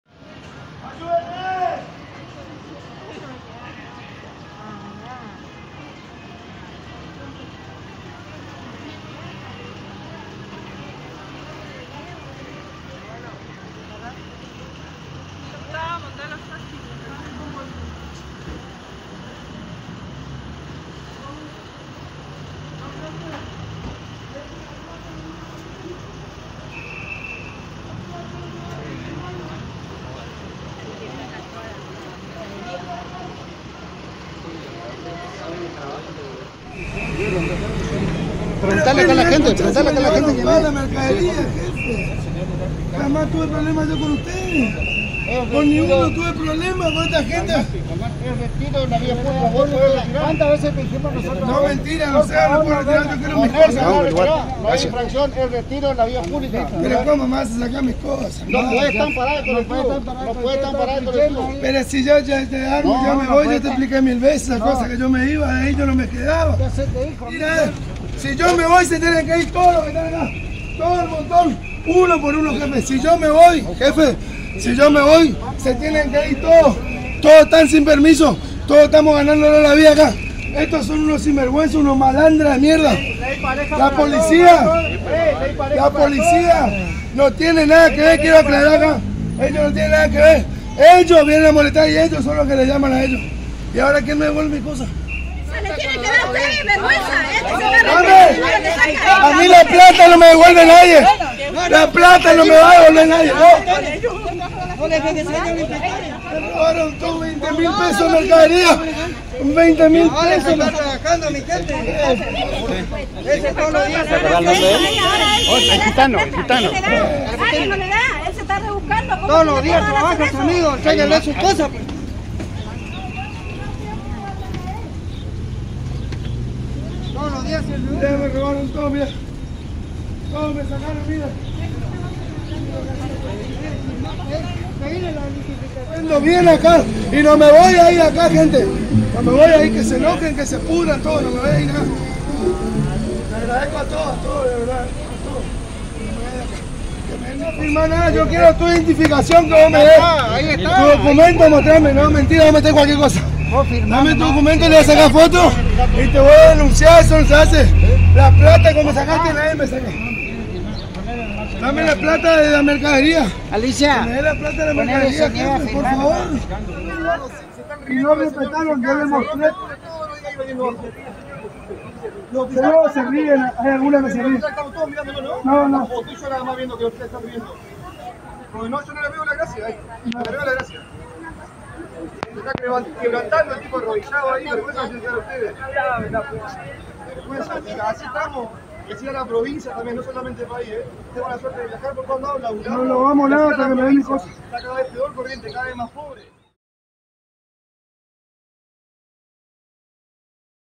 Escándalo y tensión en pleno centro con el conocido "vendedor de ajo"
Personal de la Municipalidad y la Policía de Corrientes decomisaron la mercadería del vendedor de ajo, que se encuentra en La Rioja y Junín. “Entorpece la parada de colectivos y a los transeúntes que están en la parada, hace basura y se lo intimó para que no se instale más en el lugar”, alegó uno de los inspectores municipales a Radio Sudamericana.